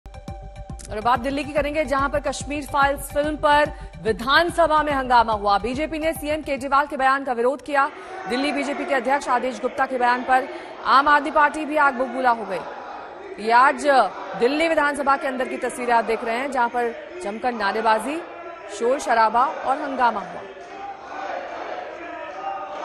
दिल्ली विधानसभा में फिर उठा कश्मीर फाइल्स का मुद्दा | Latest Hindi News
भारतीय जनता पार्टी (BJP) के तीन विधायकों को दिल्ली विधानसभा (Delhi Assembly) से बाहर कर दिया गया. बीजेपी के तीन विधायक अनिल बाजपेयी, जीतेंद्र महाजन, अजय महावर को आज पूरे दिन के लिए सदन से बाहर कर दिया गया था.